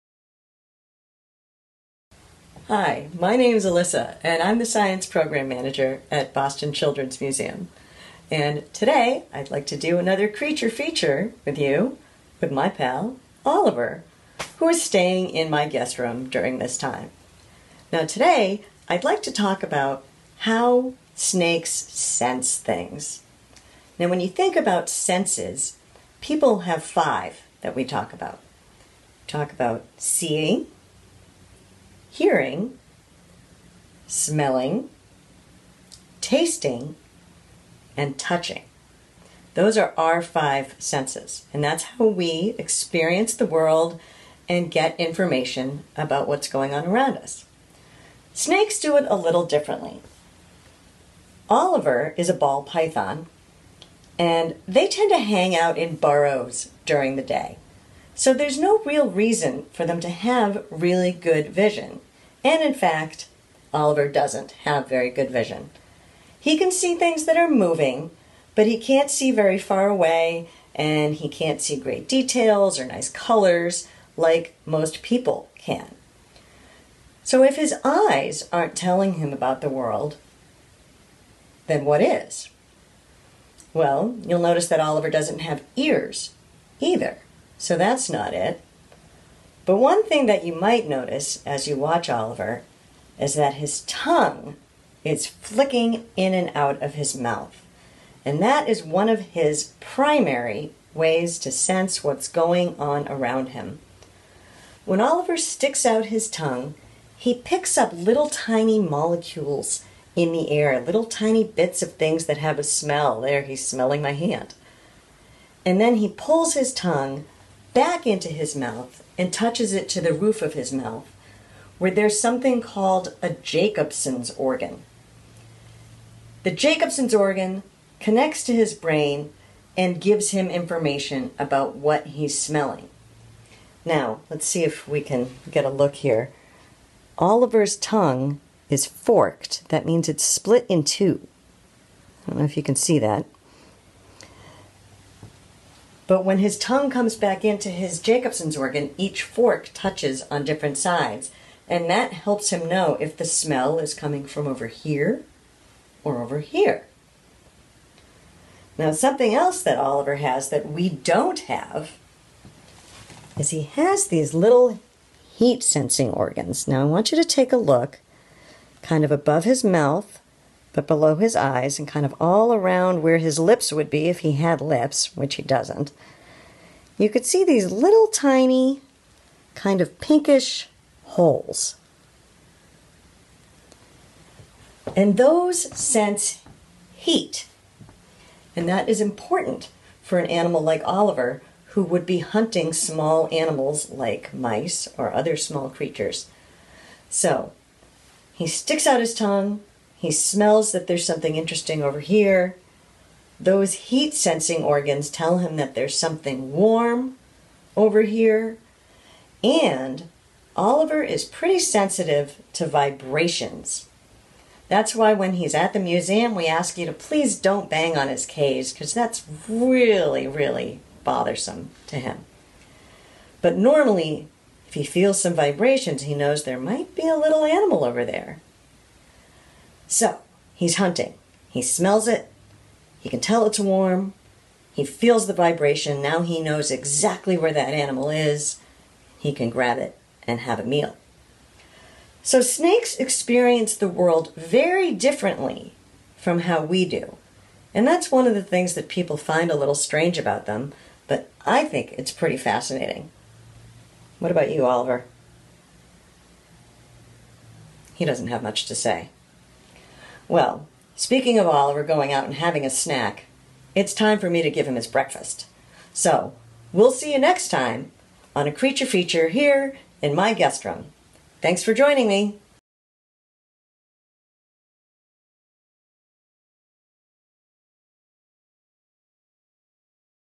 Creature Feature with Oliver the Ball Python
Let's check in with Oliver the snake, who's been hanging out at Alissa's house during the closure! Alissa is teaching us all about Oliver's senses. As humans, we have five senses — can you name them all? Snakes like Oliver, a Ball Python, have senses that differ from ours in strength and kind, like the Jacobson's Organ, for example. "What in the world is that?" you might ask...let's find out together!

Join us next Saturday, 5/23, for our first ever Creature Feature LIVE! We'll be streaming at 10am over on our Facebook page.